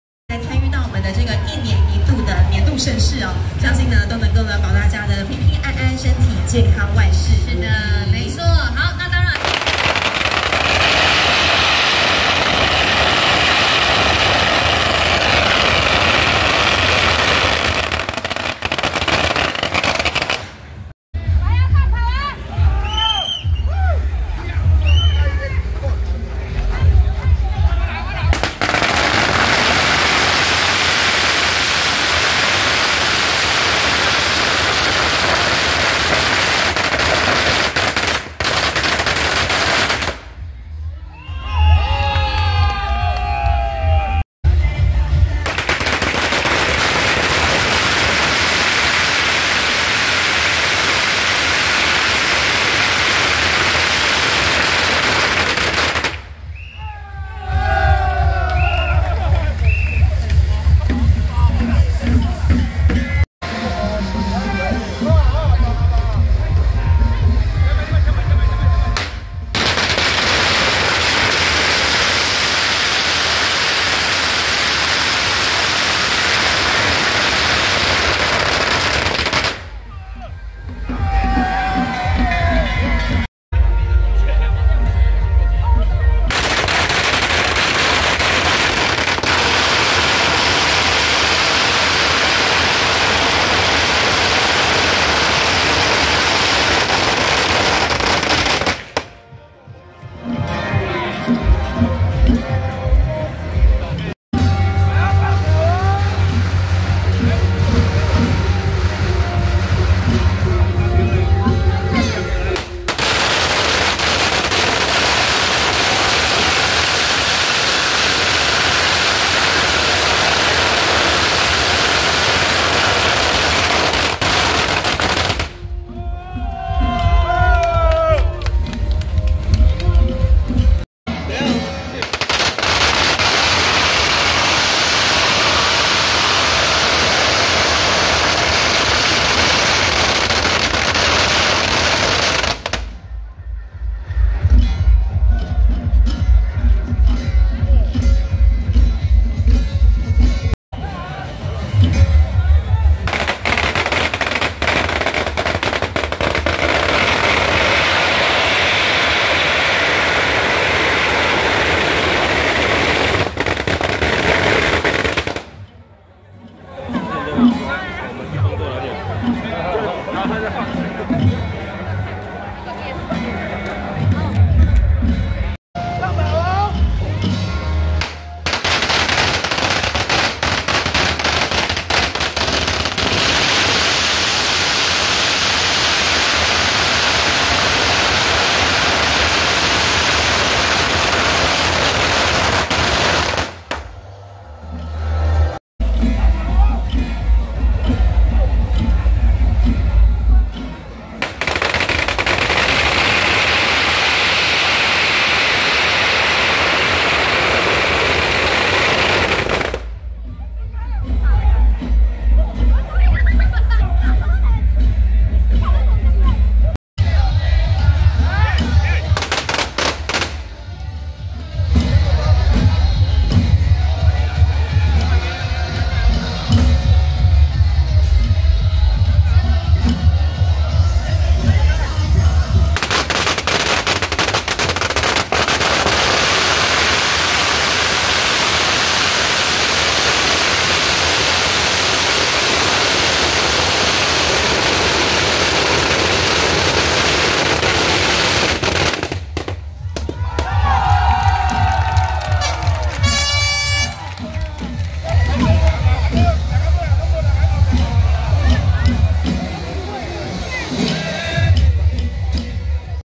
2024.05.01 朴子配天宮天上聖母聖誕遶境
2024歲次甲辰年
朴子配天宮天上聖母聖誕遶境炸轎紀錄

取材日期：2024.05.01（甲辰.三.廿三）

00:00 將軍會天德聖母炸轎
00:44 太子爺炸轎
01:03 城隍爺炸轎
01:23 六聖母炸轎
01:44 五聖母炸轎
02:10 如意媽炸轎
02:30 三聖母炸轎
02:55 二聖母轎前
03:15 大聖母炸轎
03:36 開基聖母炸轎